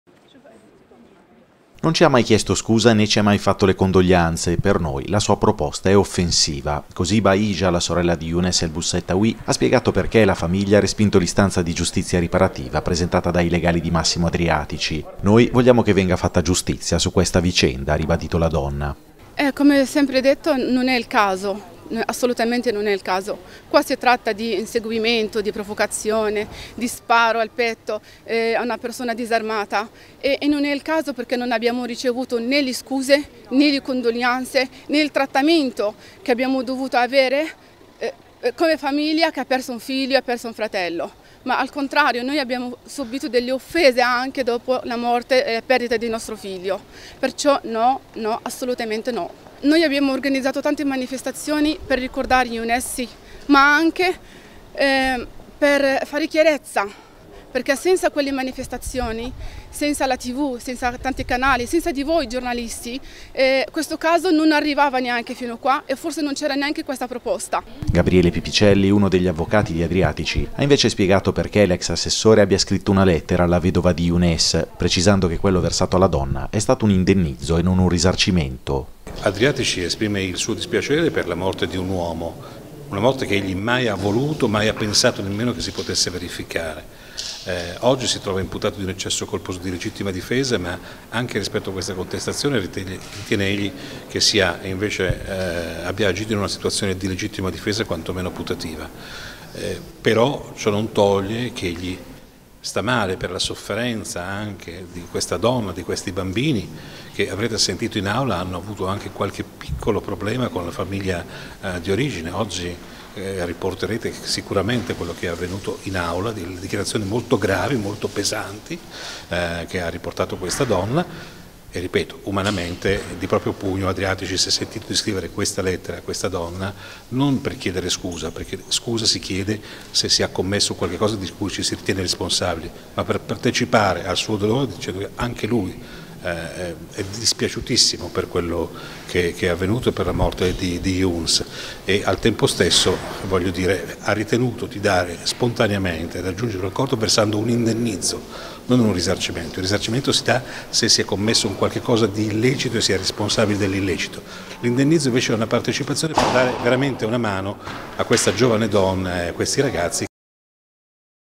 Morte El Boussettaoui, no alla giustizia riparativa proposta dai legali di Adriatici
Non ci ha mai chiesto scusa, né ci ha mai fatto le condoglianze, per noi la sua proposta è offensiva. Così Bahija, la sorella di Youns El Boussettaoui, ha spiegato perchè la famiglia ha respinto l'istanza di giustizia riparativa presentata dai legali di Massimo Adriatici. Noi vogliamo che venga fatta giustizia su questa vicenda, ha ribadito la donna.

Gabriele Pipicelli, uno degli avvocati di Adriatici, ha invece spiegato perchè l'ex assessore abbia scritto una lettera alla vedova di Youns, precisando che quello versato alla donna è stato un indennizzo e non un risarcimento.